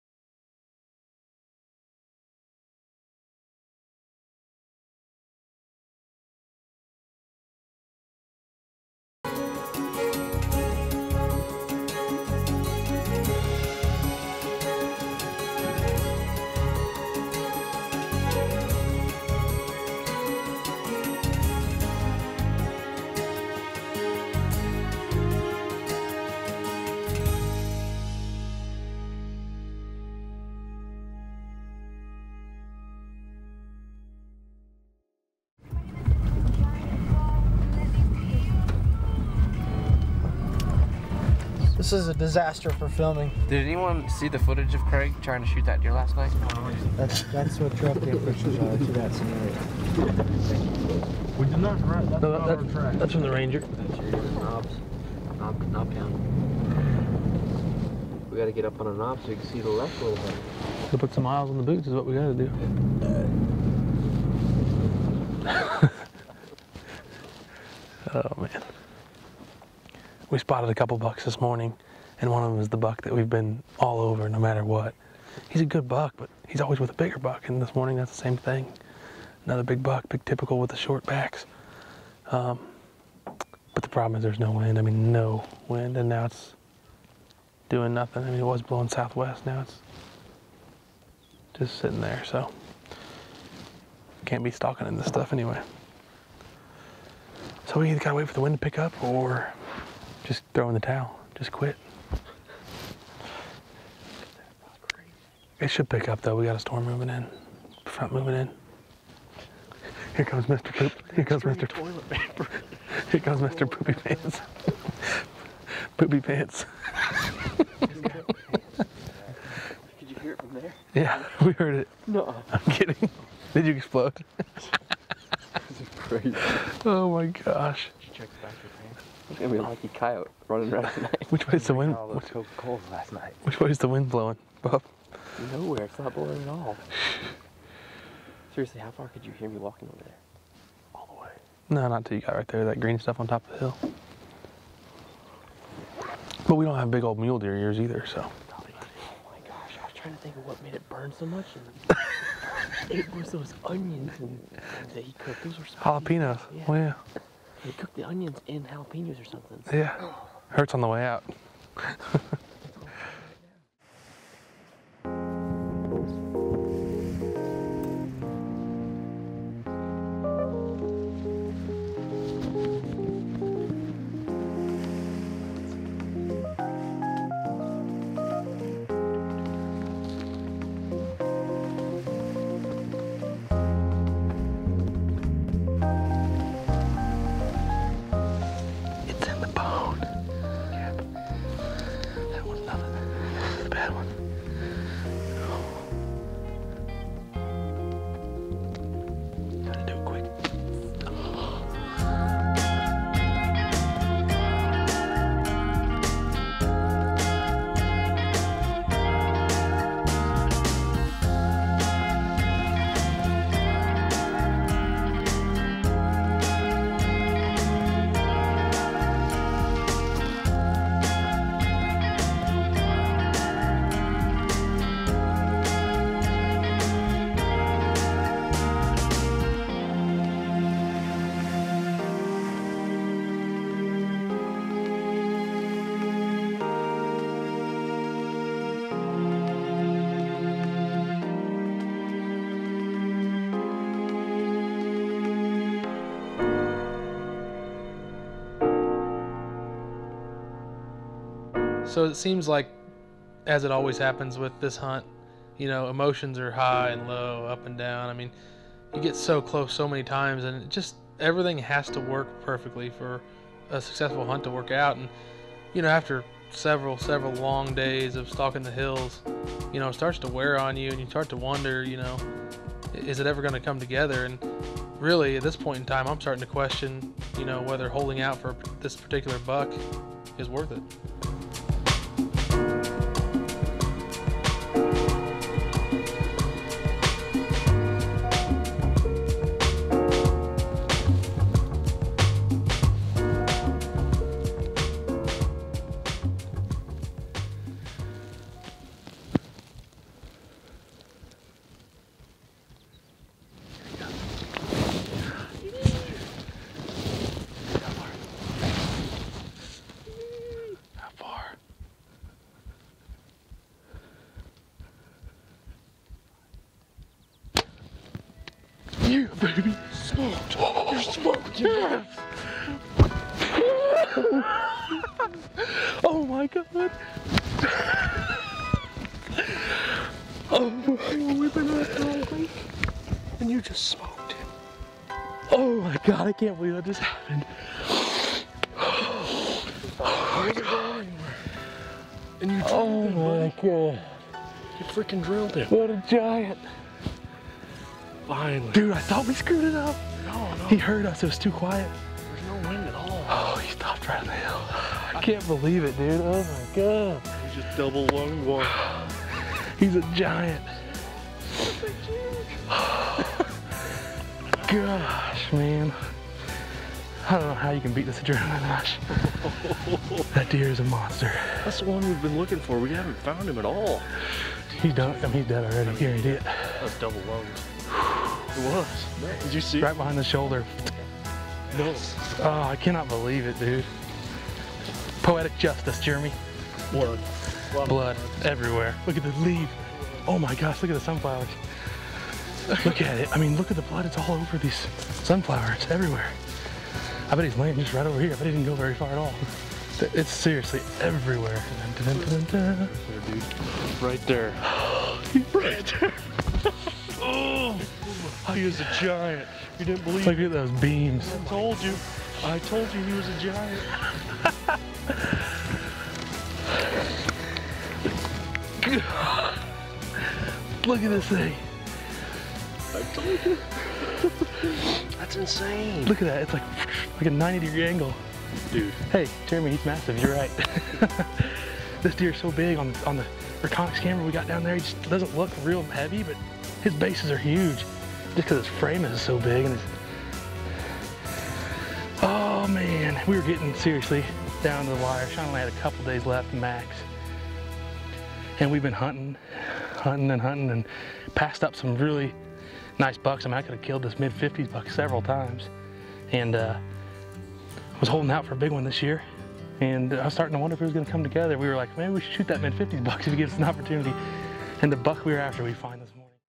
Mule Deer Hunting - 180" NEBRASKA Sandhills Mule Deer?! Perfect DOUBLE Lung With A Bow!
Still after an elusive buck nicknamed “Barbed Wire”, Mike is walking the hills of Nebraska in search for a particular deer.  This hunt from 2013 is one Mike will never forget and still goes down as his biggest Mule deer to date!